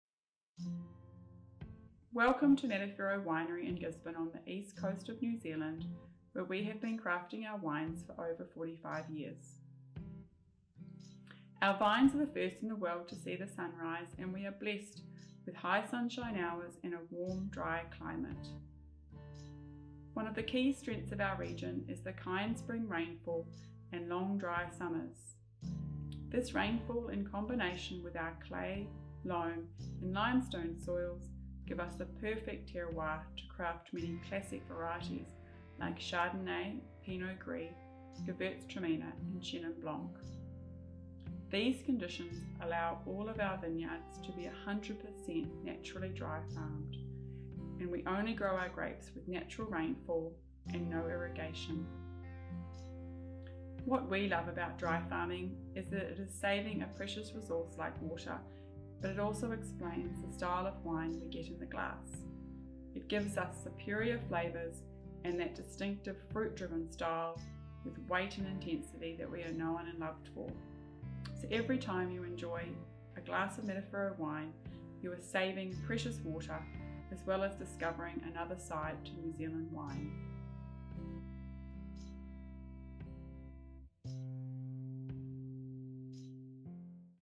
Matawhero Naturally Dry Farmed
The term dry farming does not quite conjure up the whole picture, the phrase referring to the fact that vines, while accessing water naturally, are not irrigated. With water an increasingly scarce resource in so many parts of the world, growing grapes without its addition can be both responsible and practical environmentally. Furthermore, the fruit can often benefit from this type of viticulture, with many suggesting that it yields more concentrated fruit with a stronger sense of place. All of which makes sense; when a vine is irrigated, it has no incentive to push its roots deeper into the soil in search of water. When the vines are stressed through lack of irrigation or via competition from near-by plantings, the roots will reach downwards for survival. With the Matawhero winery adherents to a dryfarming regime in sun-drenched Gisborne, one bottle of their wine saves the Earth 55 litres of water.